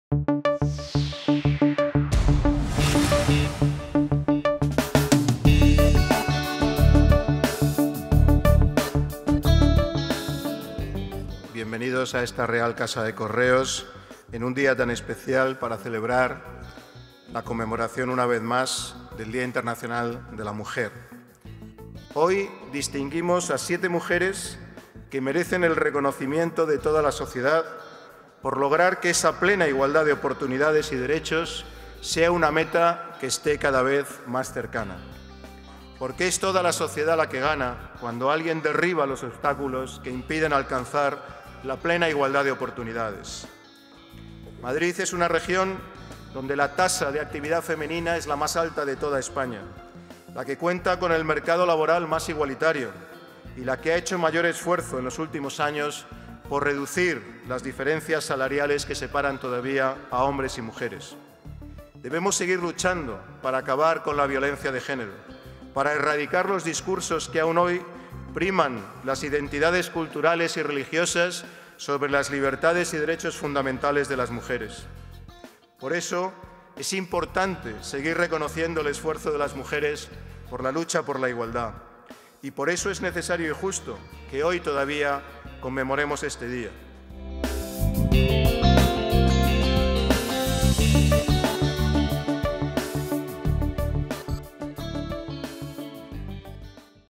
Ignacio González celebra el Día Internacional de la Mujer
El Presidente de la Comunidad de Madrid, Ignacio González, junto al Consejero de Asuntos Sociales, Jesús Fermosel, preside el acto que realiza el Gobierno regional para celebrar el Día Internacional de la Mujer, que se celebra el 8 de marzo, con el fin de sensibilizar e informar a la sociedad sobre la importancia de la igualdad de oportunidades entre mujeres y hombres, así como para reflexionar sobre el importante papel de las mujeres en el desarrollo y progreso de una sociedad.